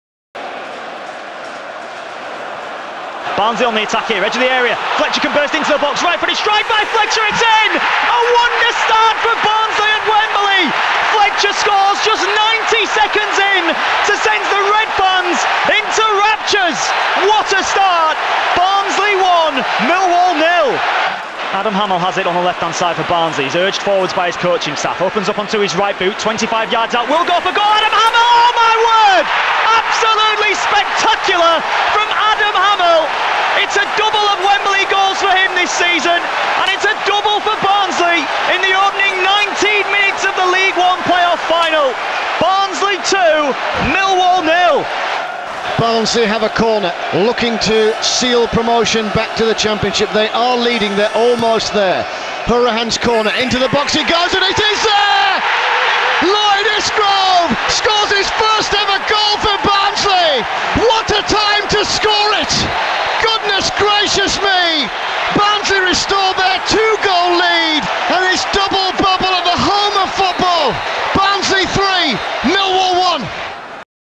Reds' Goals at Wembley (Radio Sheffield Commentary)
Enjoy the Reds' Wembley goals with commentary courtesy of BBC Radio Sheffield's Jonathan Buchan and Andy Giddings. Ashley Fletcher, Adam Hammill and Lloyd Isgrove scored the goals to secure Barnsley a return to the Championship after a sensational second half to the season.